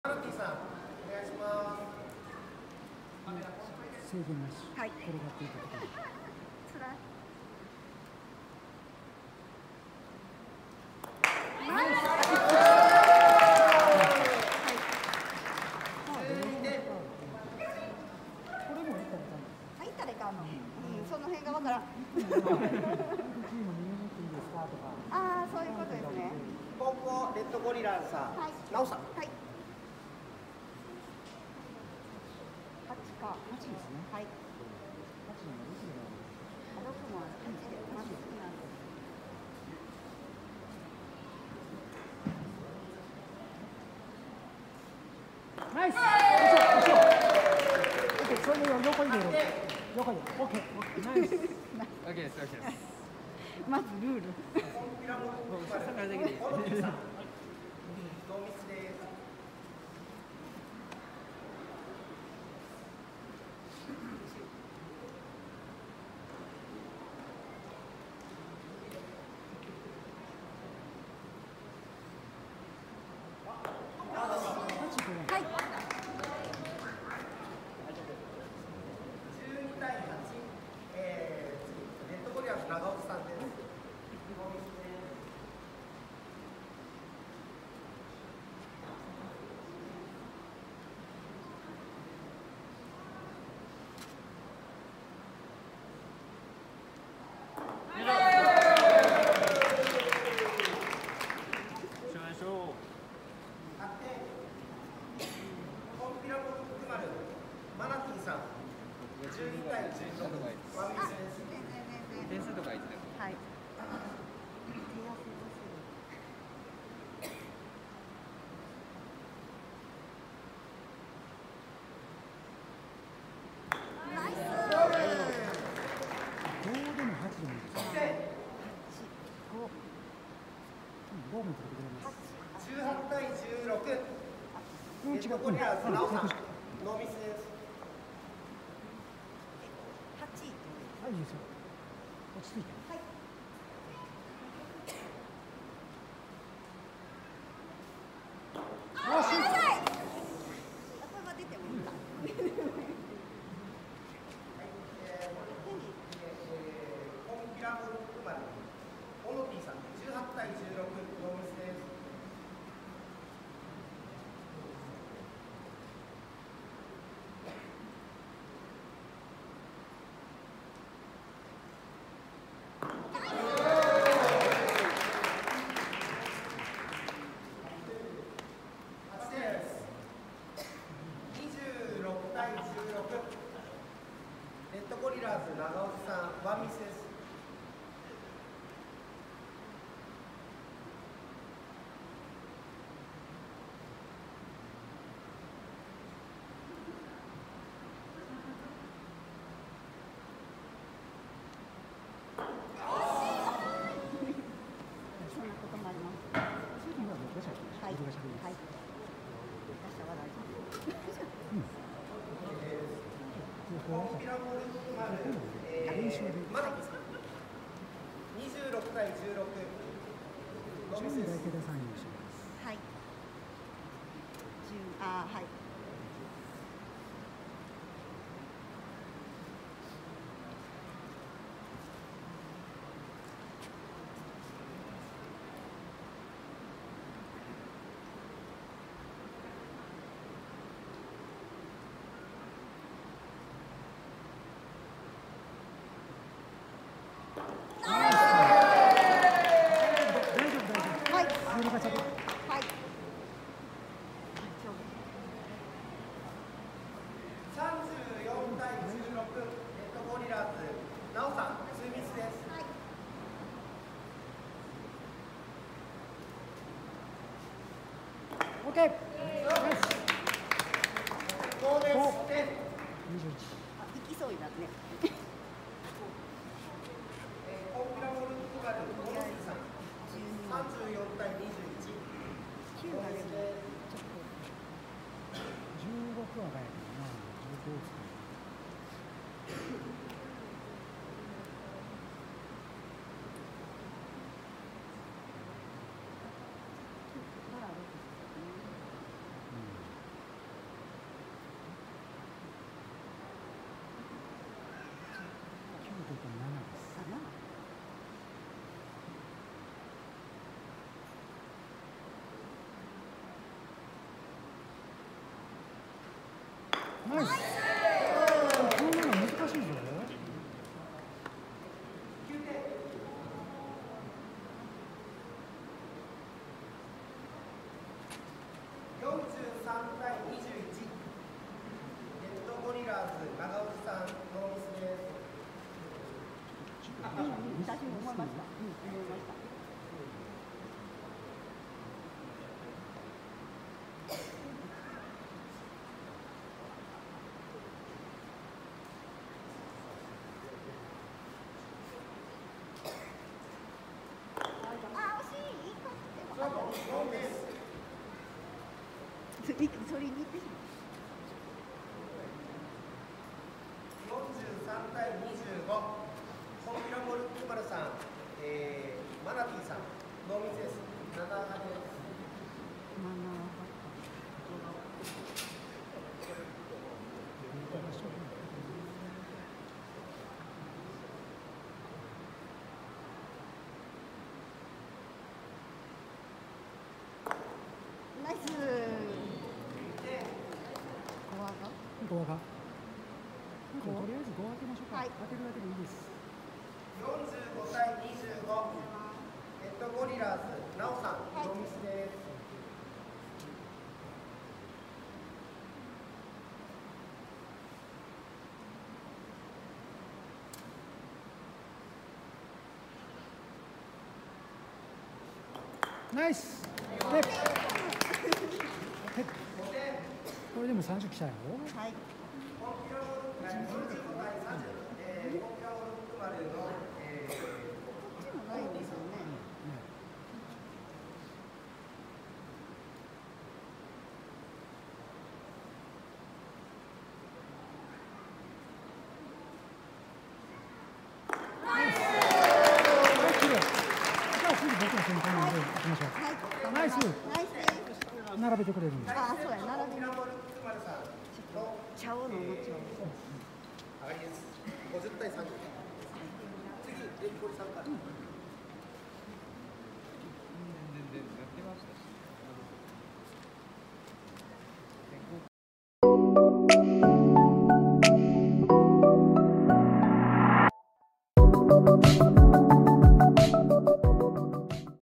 シンモルック久万高原大会 決勝戦 第1セット 編集ナシ・ノーカットver. レッドゴリラーズ こんぴらモルック丸
モルックのシンモルック久万高原大会の動画です。試合中にスーパープレイの数々や縦投げ、裏投げ、ふわり、飛ばし、ゆるガシャなどの投げ方のテクニックが見れます。この動画でコツを掴んでみてはいかがでしょうか